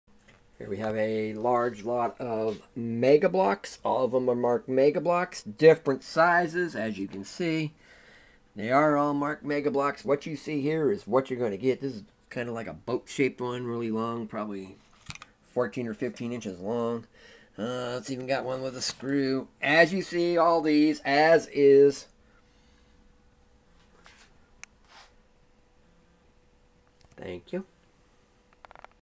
Lot of Mega Bloks Blocks Junk Box Drawer Huge used
For sale a lot of MEGA BLOKS different sizes all MEGA BLOKS. These bloks are used and in good shape. This item is for sale.